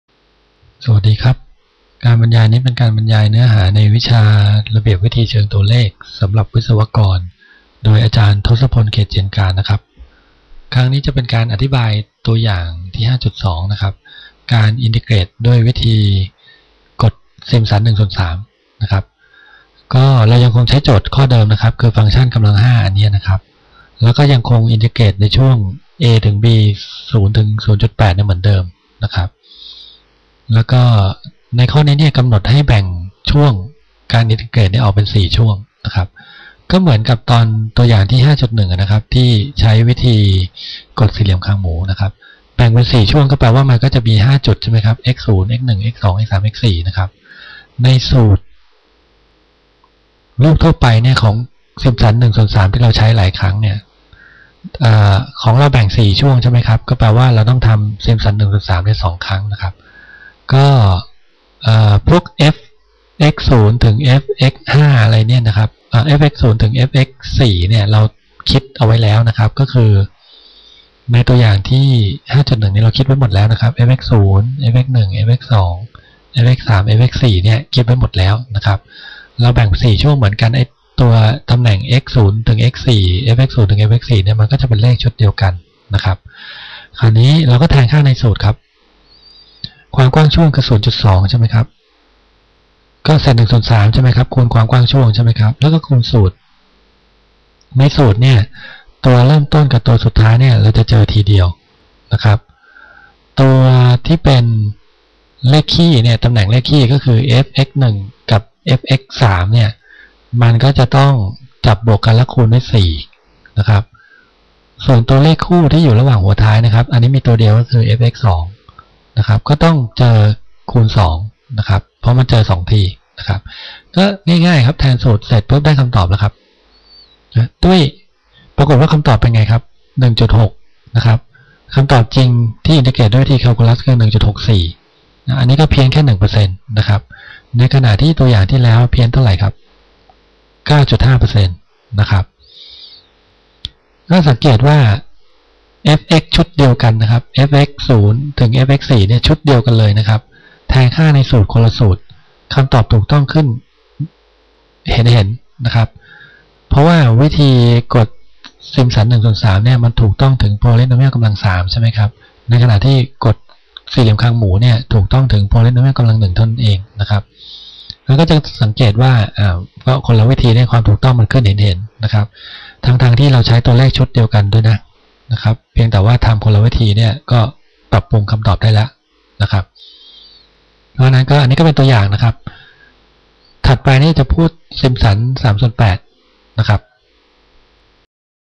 MESU Numerical Methods Ch5-5
วิดีโอชุดนี้เป็นการบรรยายเนื้อหาในวิชา ระเบียบวิธีเชิงตัวเลขสำหรับวิศวกร โดย อ.ดร.ทสพล เขตเจนการ ภาควิชาวิศวกรรมเครื่องกล คณะวิศวกรรมศาสตร์และเทคโนโลยีอุตสาหกรรม มหาวิทยาลัยศิลปากร

ท่านสามารถดาวน์โหลดไฟล์เอกสารที่เกี่ยวข้องกับวิดีโอชุดนี้ได้ที่นี่:

These "Numerical Methods for Engineers" VDOs (in Thai) were created by Dr.Thosapon Katejanekarn, Department of Mechanical Engineering, Faculty of Engineering and Industrial Technology, Silpakorn University, THAILAND.